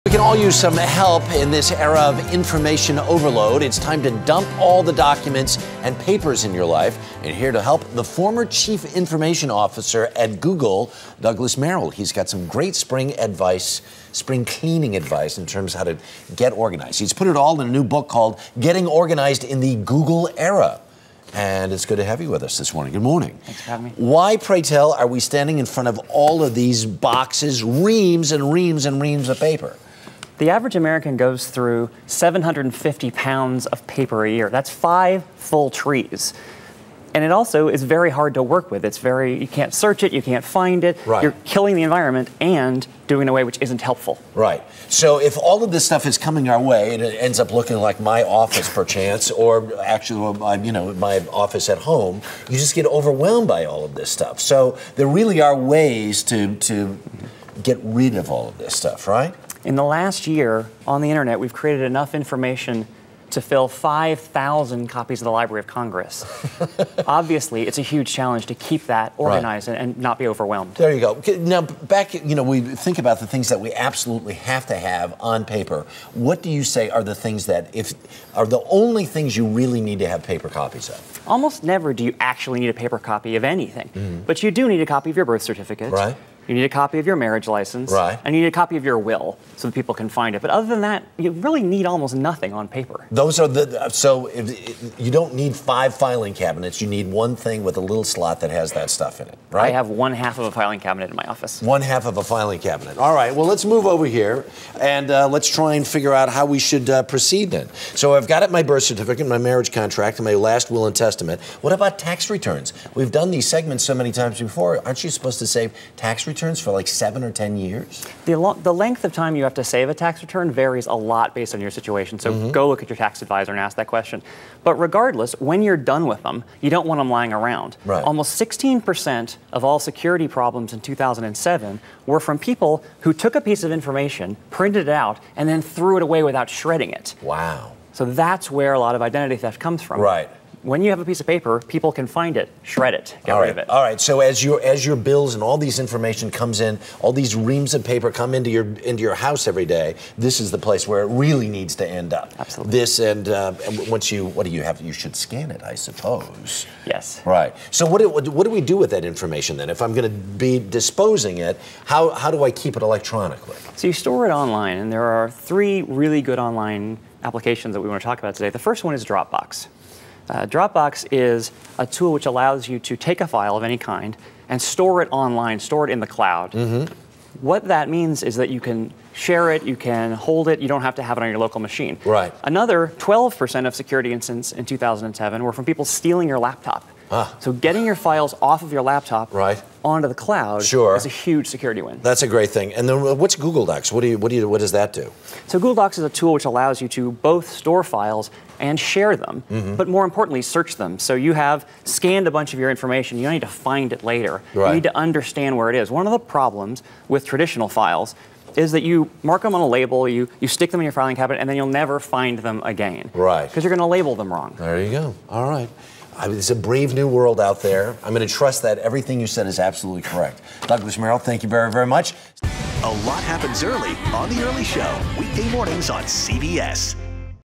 Paperless Data Storage
Douglas Merrill, author of Getting Organized in the Google Era, on moving away from hard copies of data and storing information in the cloud.